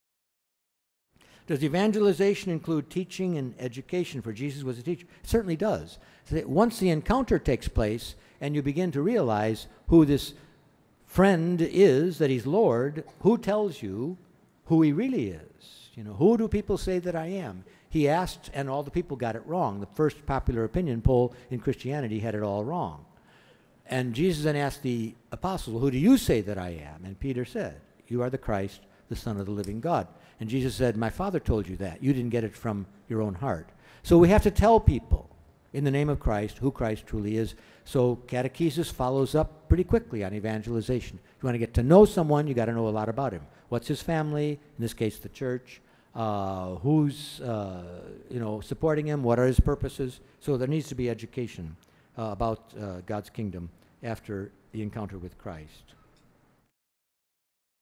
Cardinal George on... Catechesis
Cardinal Francis George answers questions at Parish Leadership Day 2009 at Maria High School.  Francis Cardinal George talks about catechesis